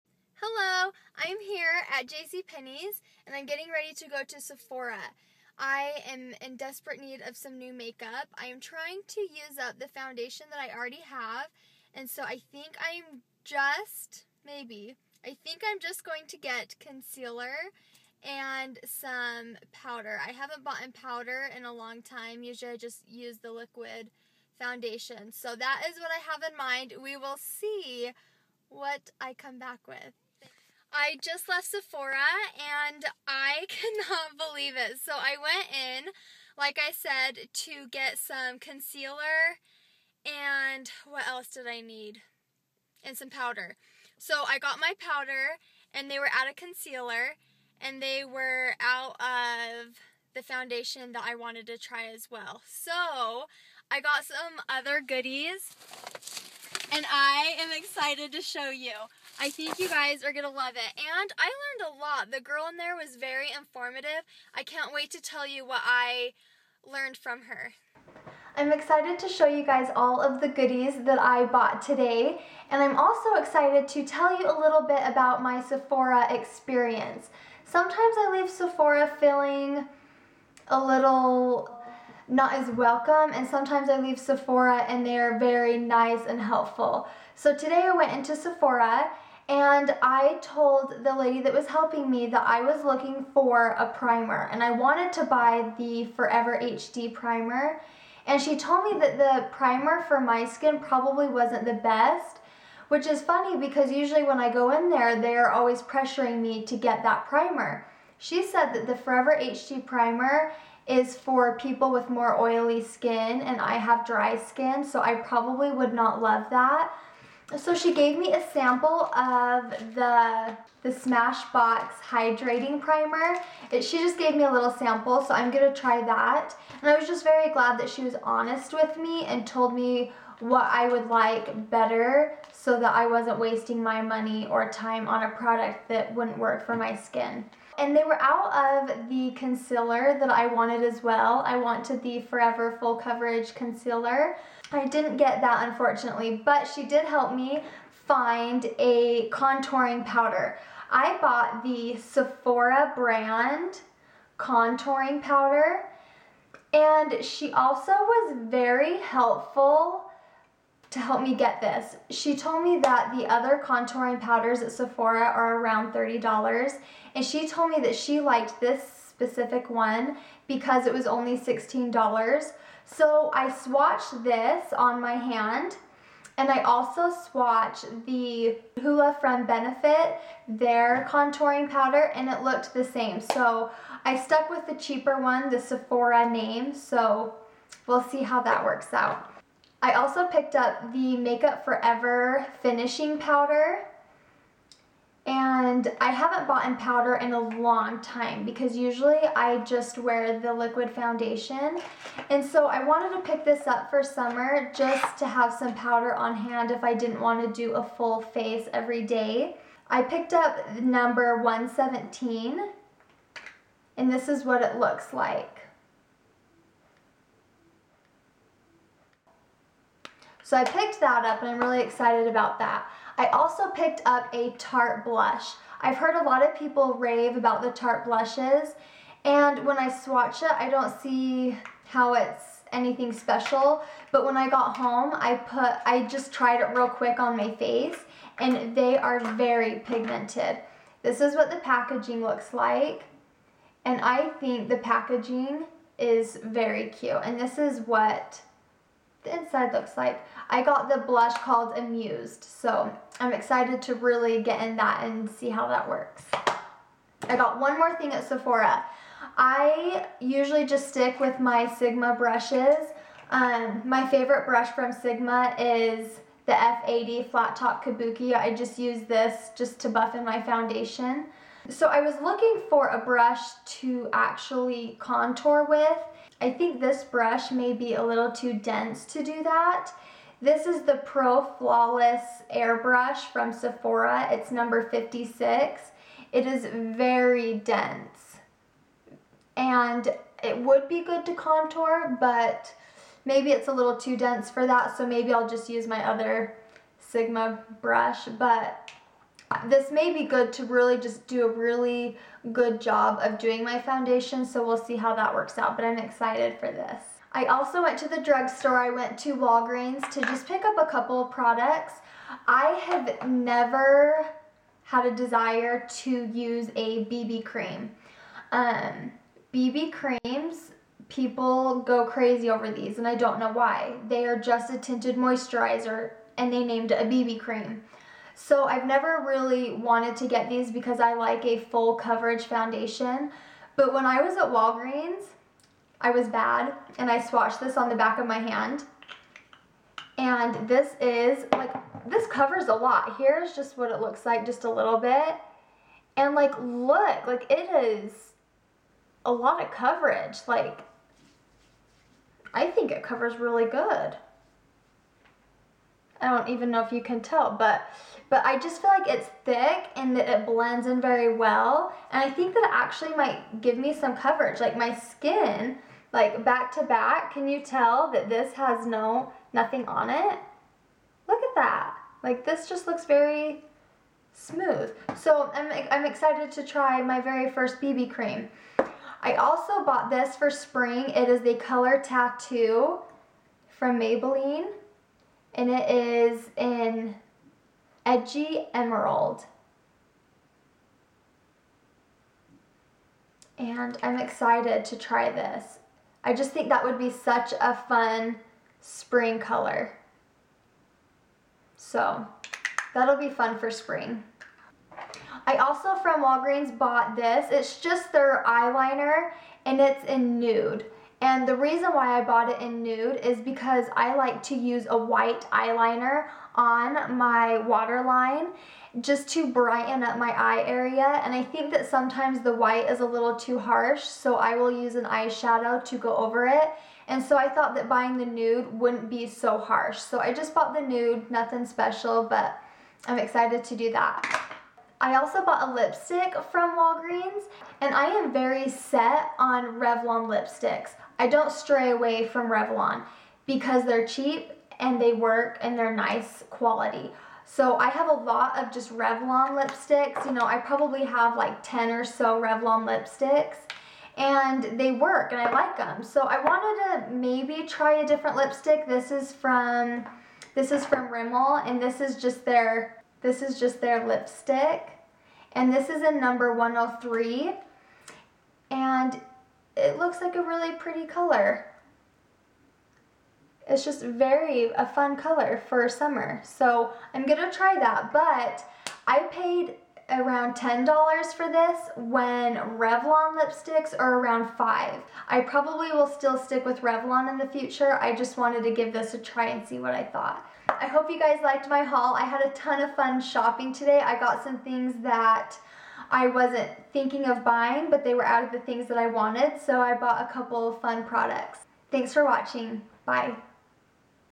Makeup Haul: Sephora and Walgreens!
Everything I bought!
Makeup Forever Pro Finish Powder/ #117, switched it for #113
Sephora Pro Finish Air Brush/ #56
Tarte Blush/ Amused
Rimmel Eyeliner/ Nude
Rimmel BB Cream/ Light
Color Tattoo Maybelline/ Edgy Emerald
Sephora bronzer/ los cabos #3
Rimmel Lipstick/ #103

Hope you liked the video! Let me know if there is anything you would like to see and I will get it done! :)